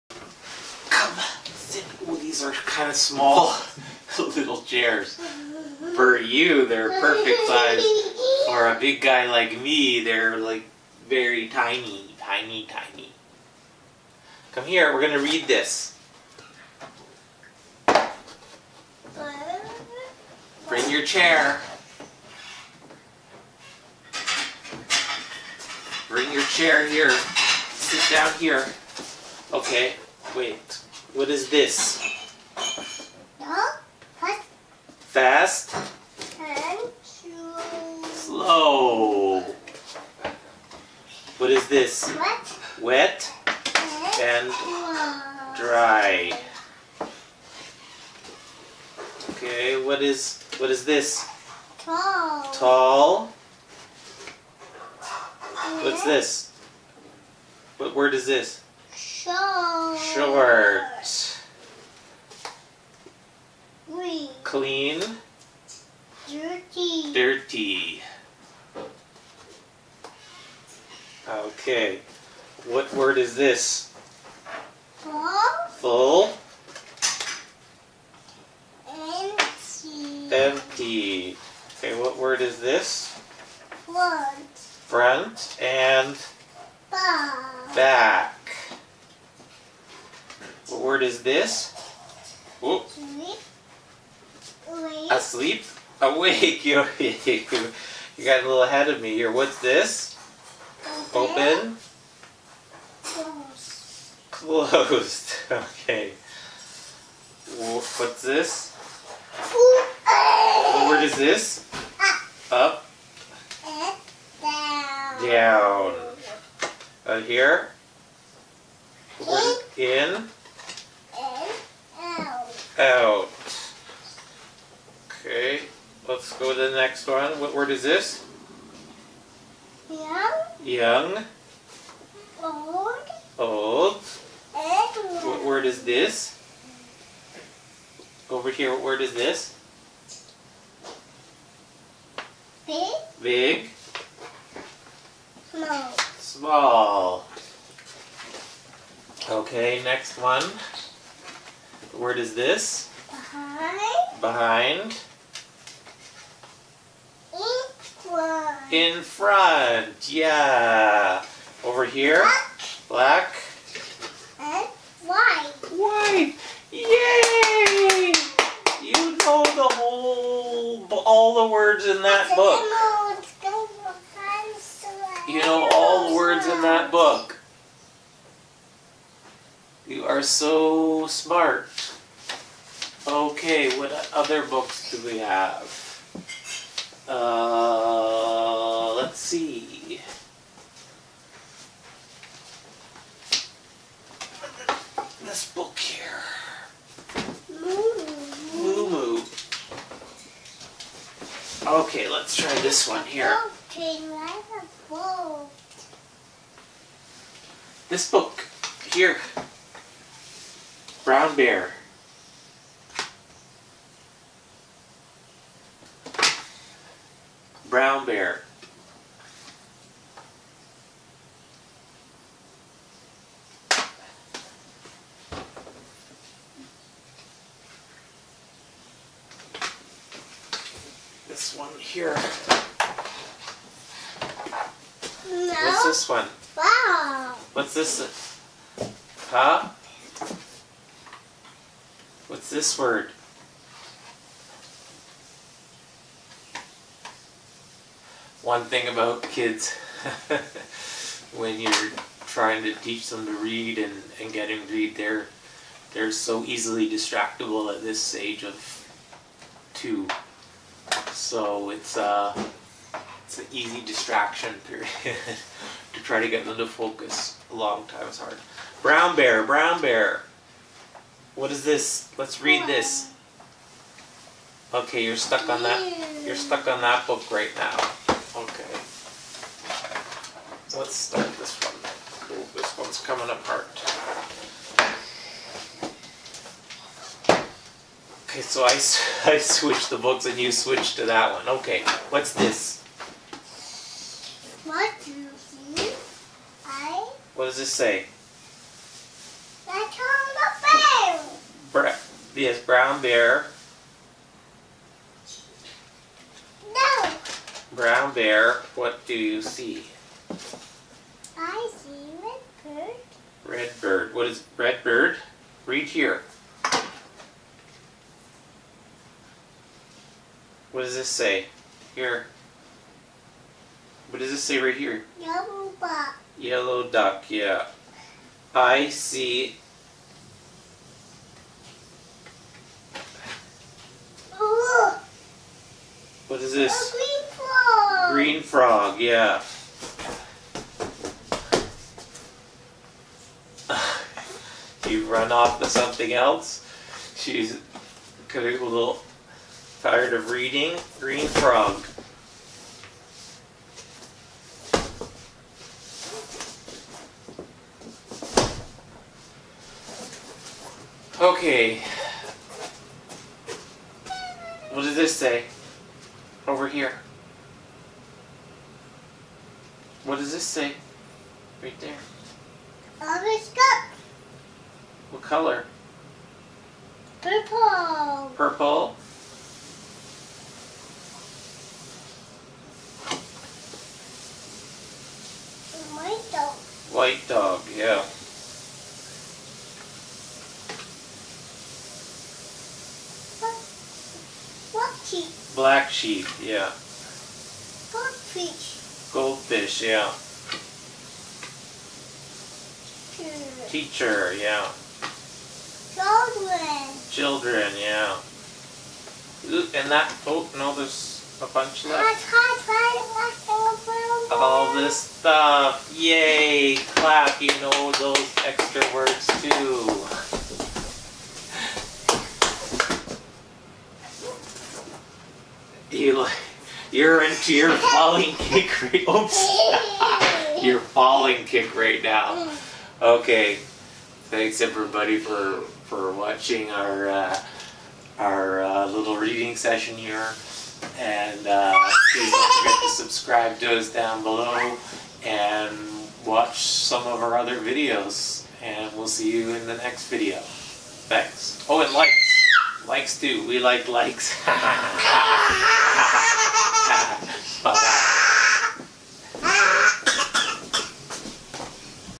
Toddler Reading - opposite words
Emily has actually been reading before 2 years old. We are just doing some practicing in this video. My wife has been amazing at teaching her how to read. One of the really easy and quick things to teach your child is the opposites. They can pick up on the opposites quite easily. Of course learning the opposites come first, then actually be able to read the words.
Of course keeping a 2 year old focused on any task for too long is a real challenge!

2 year old reading, learning to read, toddler reading, easy reading, how to teach reading, 2 years old, phonics, books